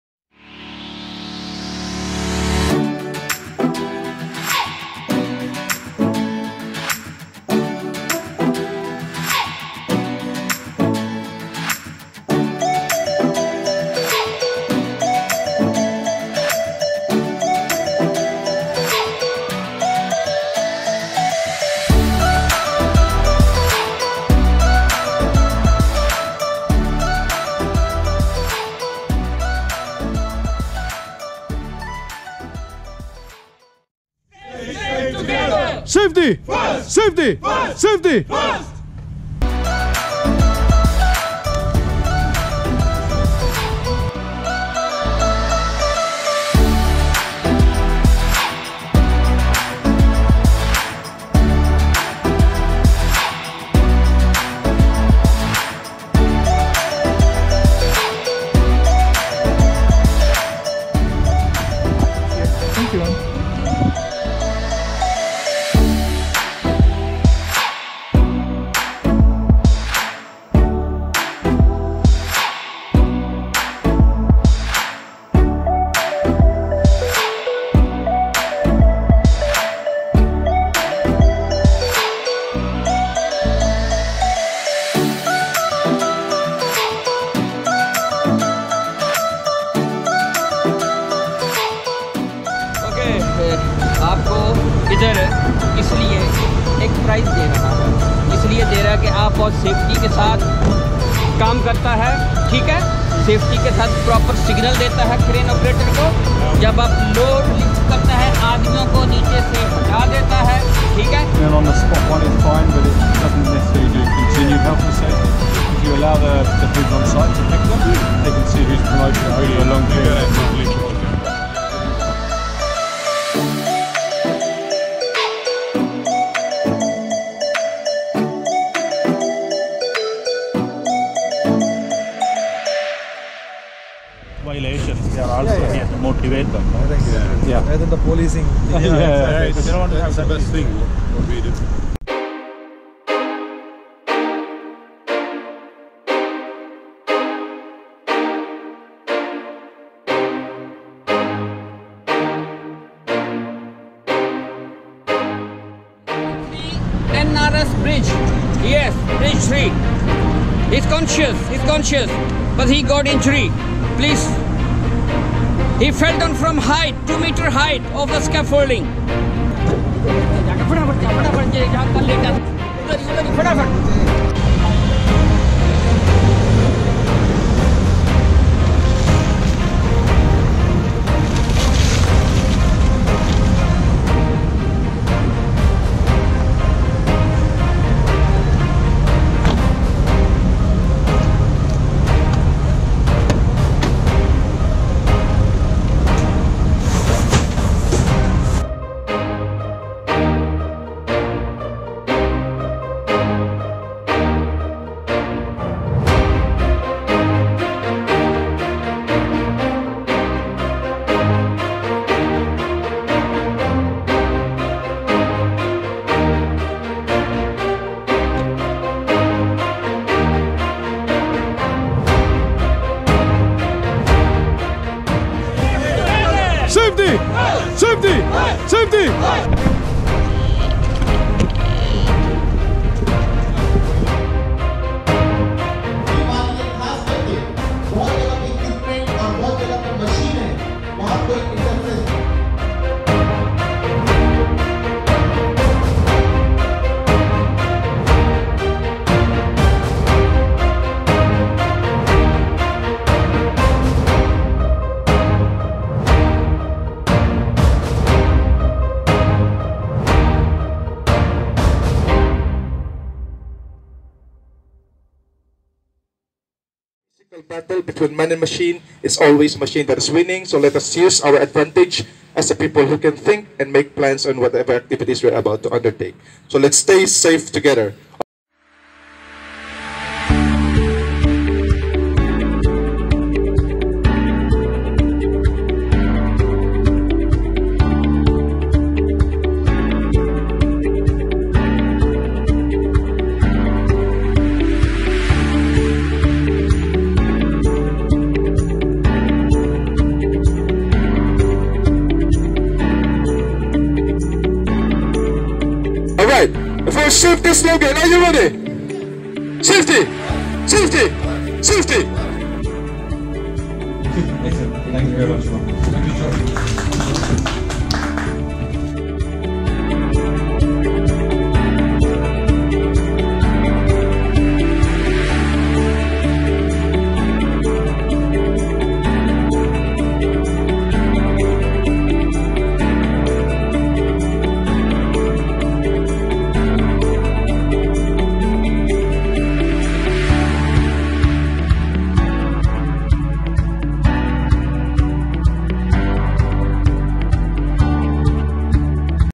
Stay Safe Together Campaign 2019
A WSP Lead Campaign with the participation of Daewoo E&C as the Construction Contractor of Ashghal's Construction and Upgrading of E-RIng Road Project. This is to continually improve the level of safety culture within the organization under the Supervision of WSP.